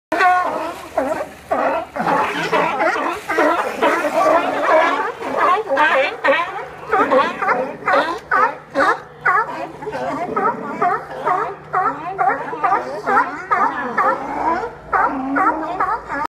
Sea Lions in Newport Oregon
It's fun to go see the sea lions battling for a spot or just barking on the docks down on the waterfront in Newport Oregon.
Directions: If you want to see them you need to go down to SW Bay Blvd. There is a board walk on the right side (West) of Undersea Gardens which is at 250 SW Bay Blvd. Newport, OR 97365.